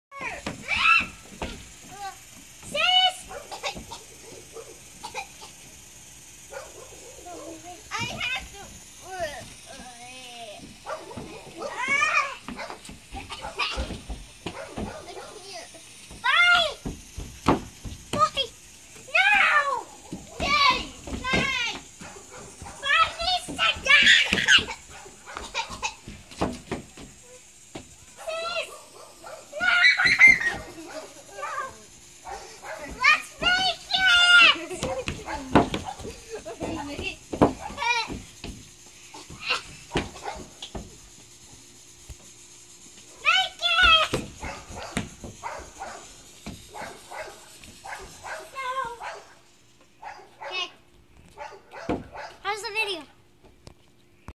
THE FOG MACHINE WITH FOG CHILDREN
My girls are enjoying the Pre-Halloween  with The Fog as we play out side the house. My girls think they are in the movie with a lot of FUN. Your Truly BUFFY. XOXO PLEASE TELL ME WHAT YOU THINK??